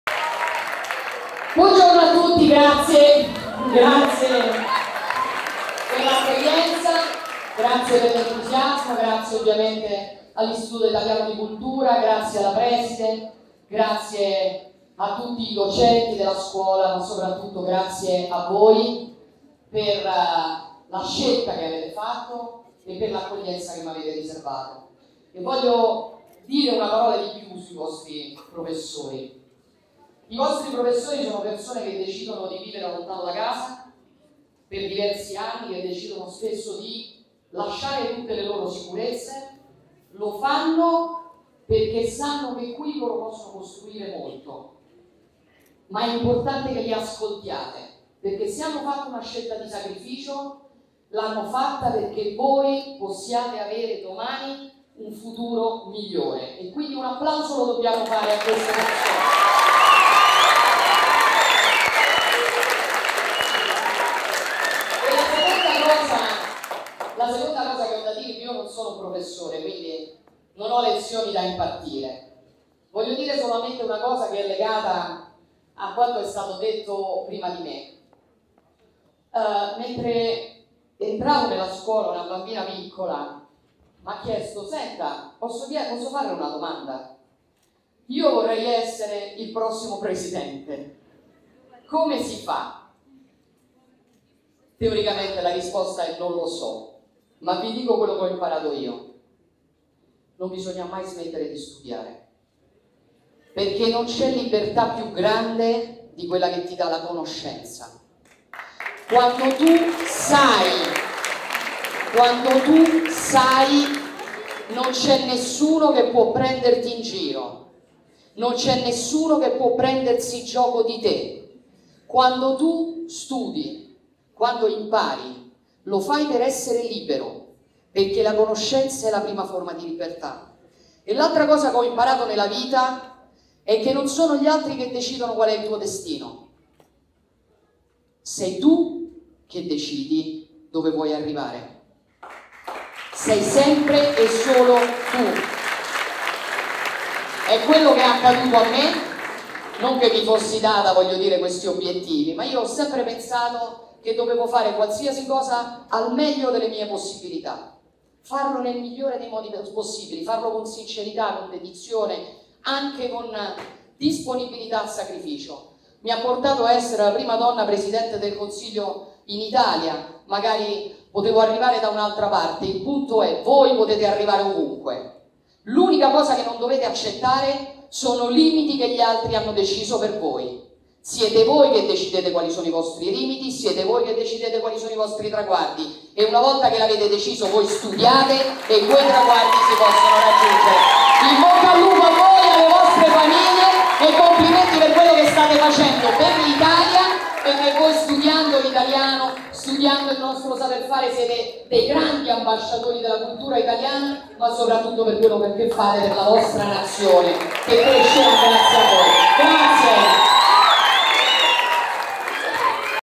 Meloni - Addis Abeba, il Presidente Meloni interviene alla Scuola italiana (15.04.23)
- Meloni - Addis Abeba, il Presidente Meloni interviene alla Scuola italiana
Addis Abeba, 15/04/2023 - Il Presidente del Consiglio Giorgia Meloni interviene alla Scuola statale italiana "Galileo Galilei". (15.04.23)